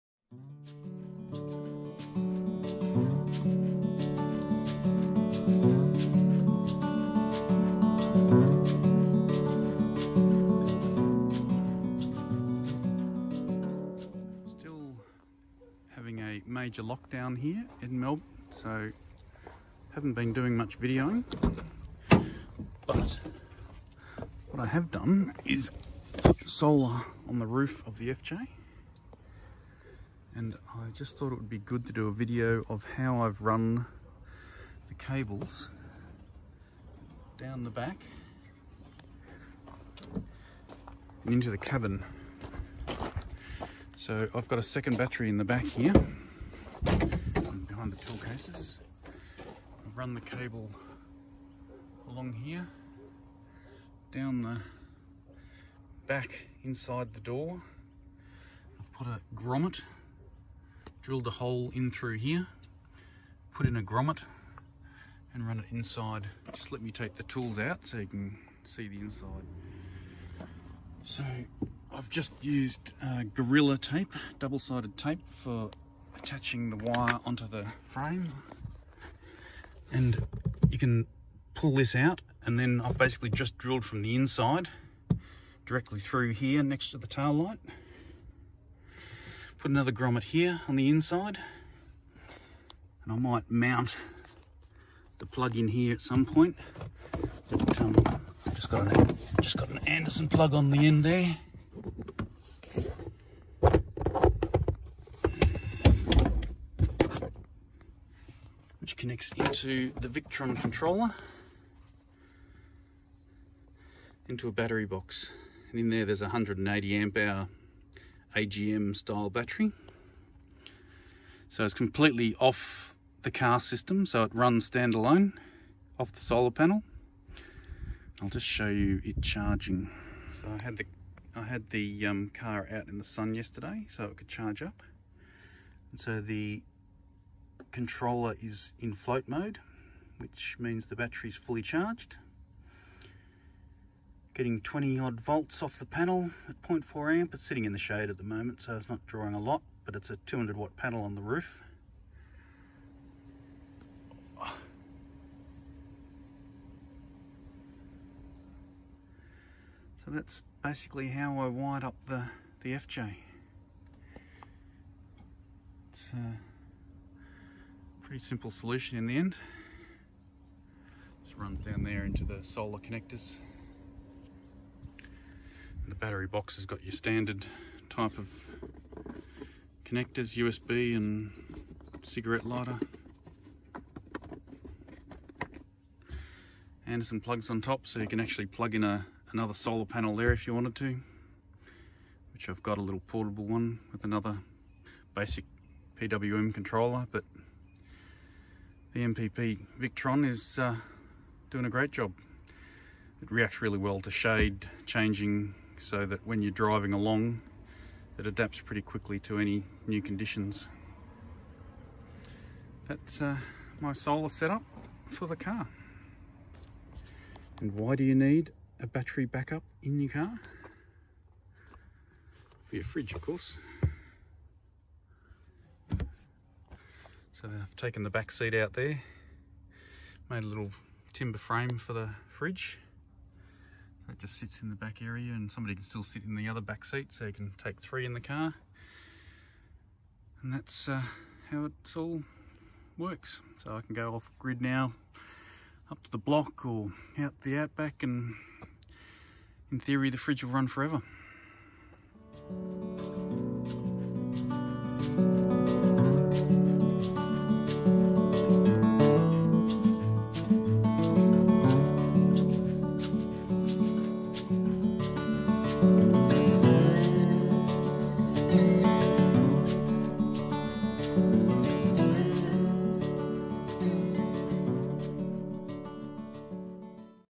Toyota FJ Cruiser Solar Panel Set Up and Cable path to rear Cabin
Another lockdown, another solar project. This time it is the FJ Cruiser's turn. Using 50 amp car electrical cable I have run from the solar panel on the roof rack to the rear cabin where I have a 180 Ah AGM Battery to power the 50 liter fridge. In theory this should run the fridge indefinitely and not impact the car battery in any way.